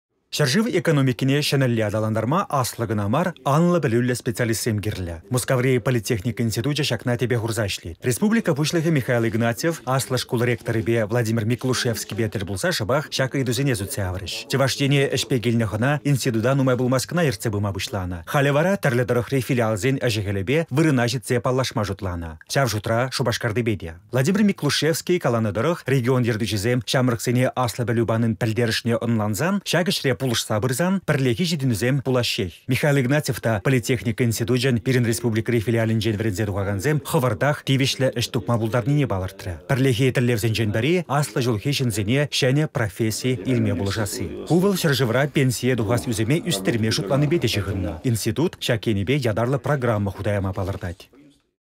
Аслă пĕлӳллĕ çамрăксем йышланĕç
Çĕршыв экономикине çĕнĕлле аталантарма аслă кăна мар, анлă пĕлӳллĕ специалистсем кирлĕ. Мускаври политехника институчĕ çакна тĕпе хурса ĕçлет. Республика Пуçлăхĕ Михаил Игнатьев аслă шкул ректорĕпе Владимир Миклушевскипе тĕл пулса шăпах çак ыйтусене сӳтсе яврĕç.